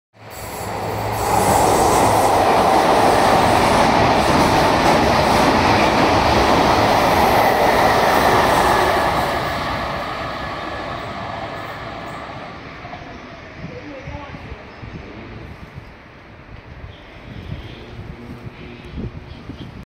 Just in time! Thalys flies through Lelylaan - Short Fly-by -
Almost let this one slide, but luckily I looked behind me and saw the oncoming Thalys, while I was strolling under the canopy of Amsterdam Lelylaan.

Didn't hesitate for a moment, took a few steps from other passengers and took the shot, just as long as I could before other passengers would block the image, in this very unflattering light!

- Wednesday, the 16th of November, 2022